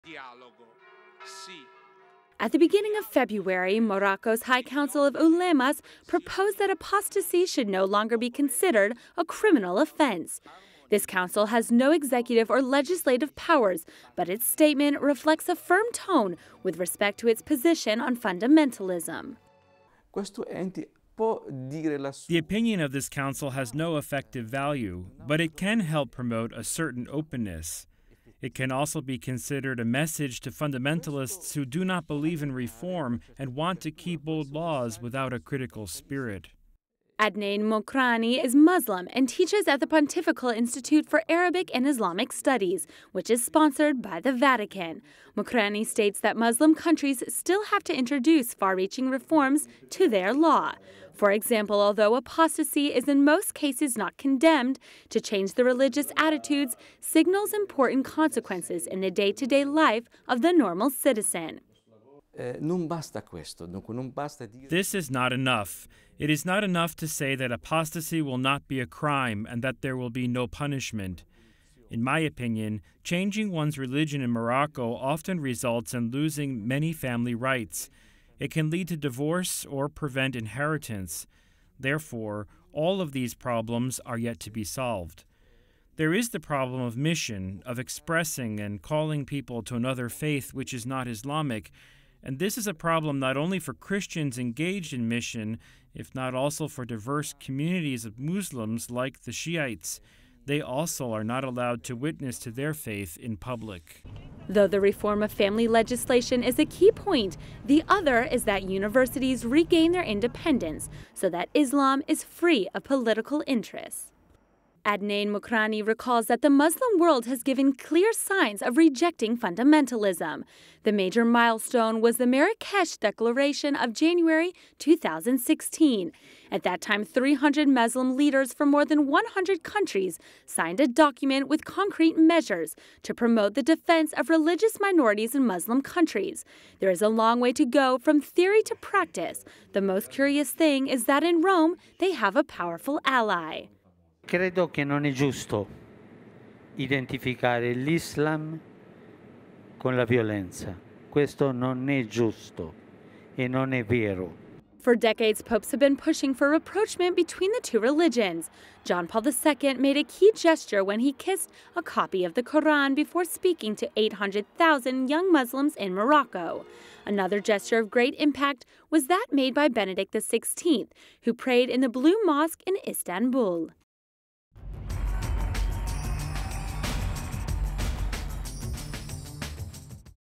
Pontifical Institute for Arabic and Islamic Studies: Muslim world shows signs of change
March 25, 2017. “Fundamentalists do not believe in reform. They want to keep old laws without a critical spirit.”